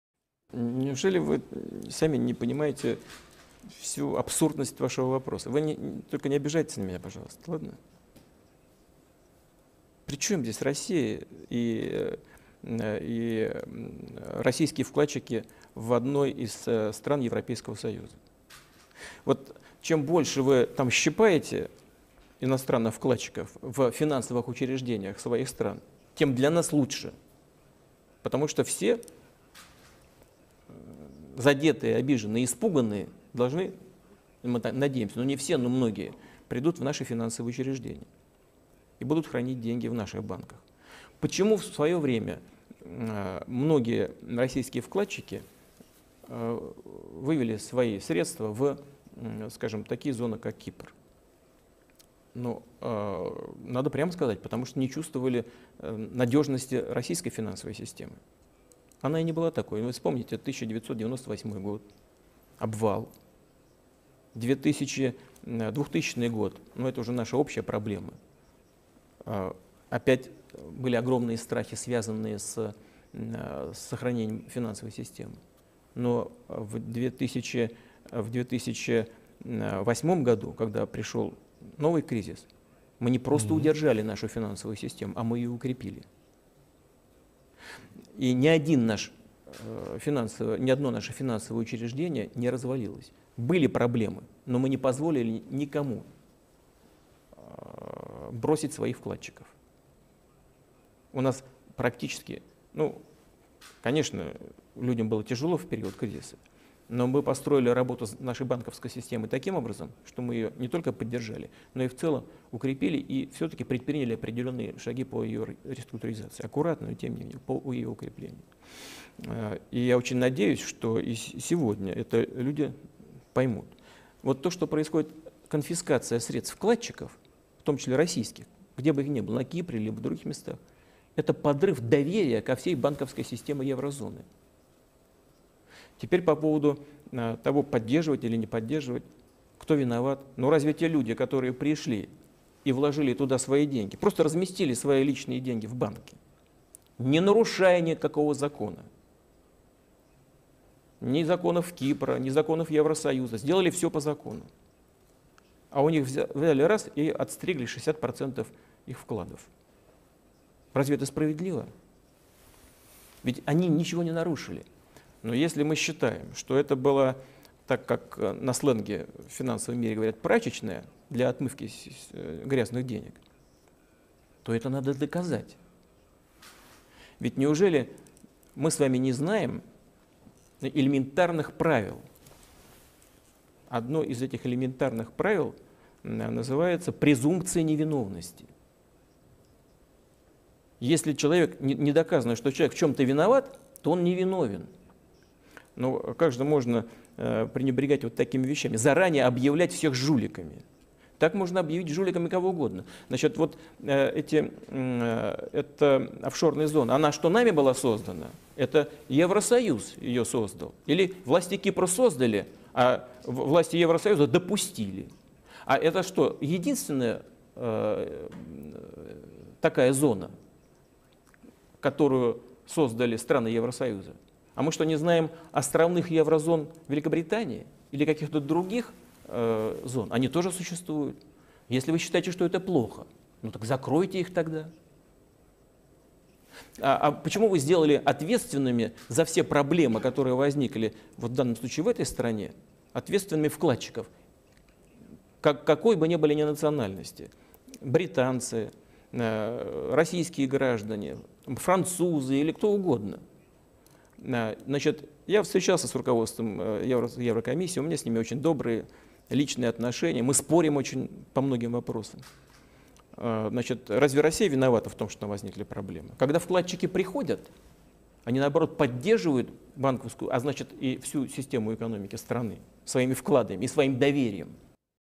Путин - Российский !!!ОФШОР!!! на Кипре
6ой вопрос из Интервью В.В.Путина немецкой телерадио-компании «ARD» 6 апреля 2013. !!!Почему Россия УВОДИТ ДЕНЬГИ В ОФШОР!!! Данный вопрос, по мнению либеральной прессы должен был, как минимум сконфузить Путина, как максимум заставить оправдываться... ОДНАКО на деле всё вышло с точностью до наоборот ;)
Опять Путину пришлось поучать неразборчивых рукопожатых журналистов...